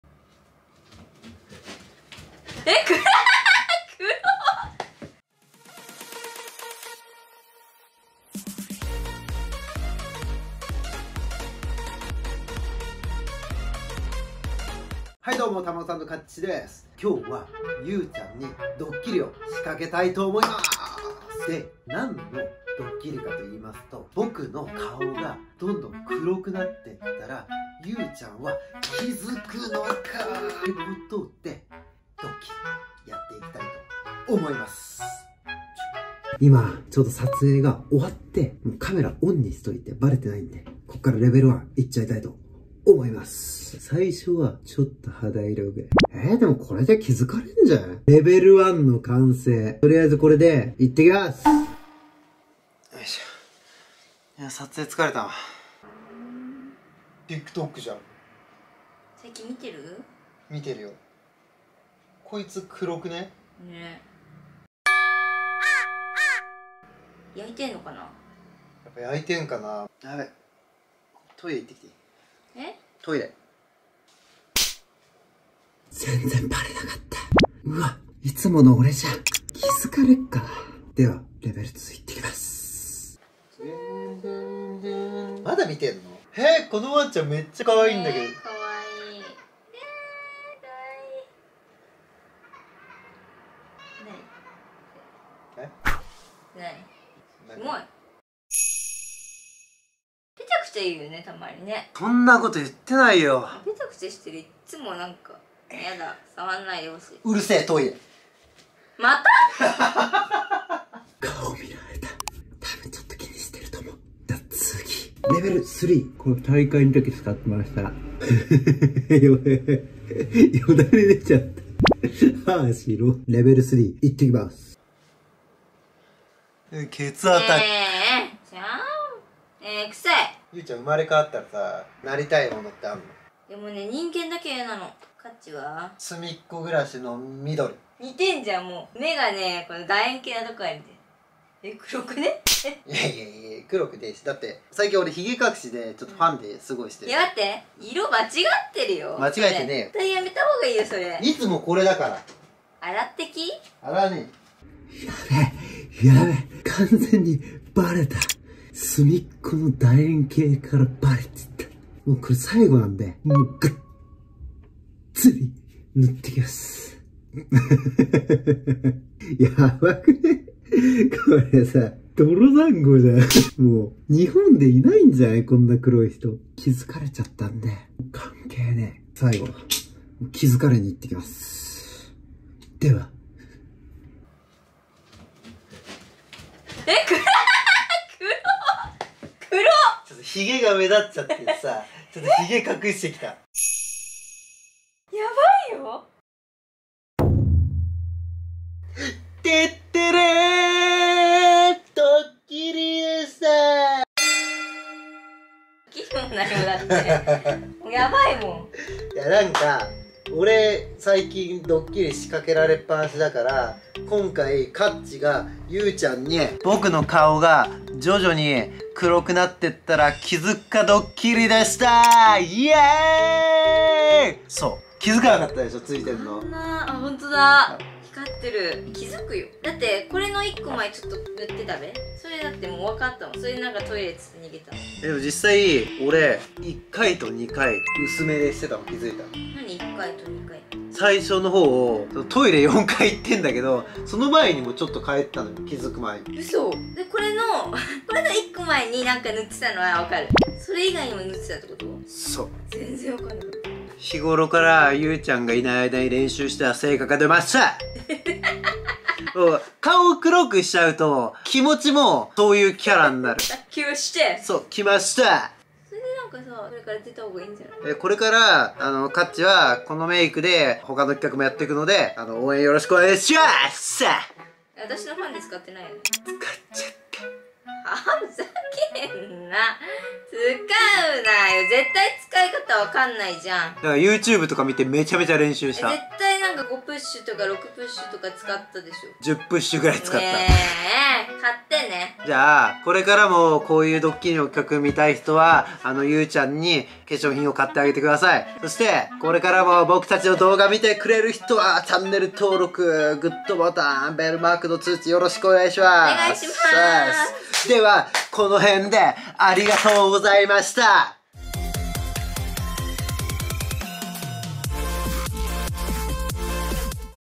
【検証】顔が徐々に黒くなっていくドッキリwwえ！顔黒くね？ってなるのか？
かっちの顔が
黒くなっていったらゆーちゃんは気づくのか？

徐々にかっちの顔が
黒くなってくるんだけど
みんなは気付けるかな？ww

『青春を取り戻す！』をテーマに様々な企画を沢山動画にしていきたいと思います。

全力疾走で駆け上がります。